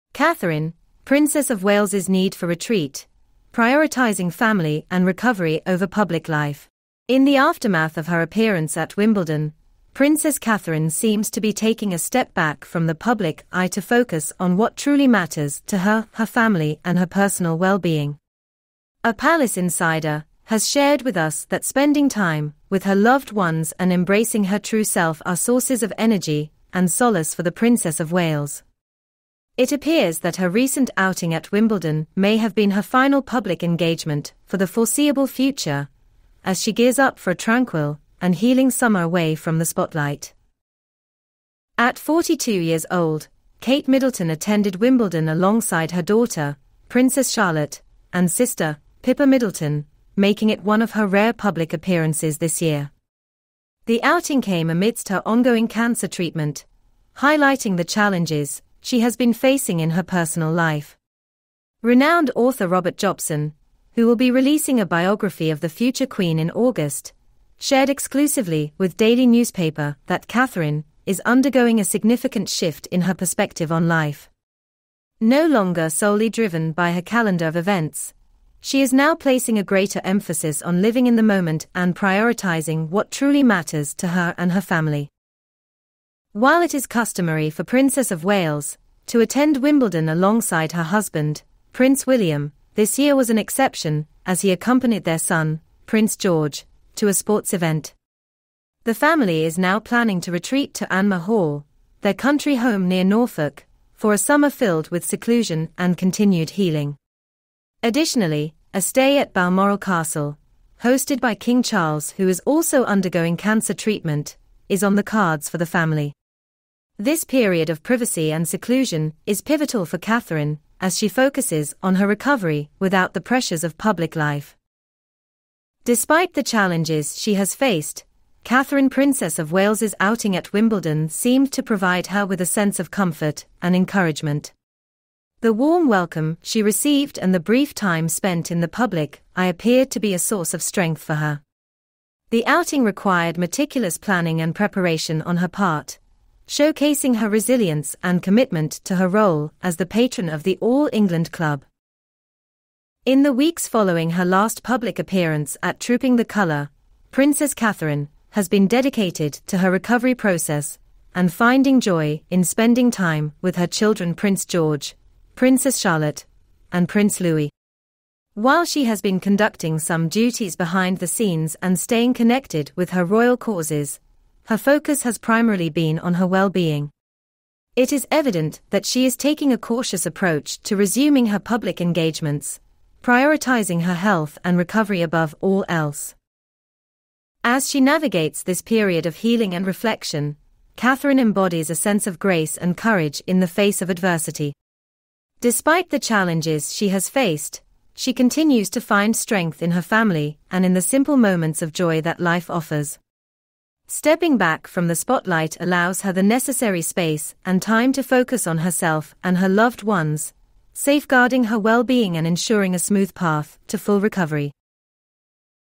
Palace Kensington Update! Princess Catherine Need a Retreat Prioritizing Family over public life.
Palace Kensington Give Rare Update! Princess Catherine Need for RetreatPrioritizing Family .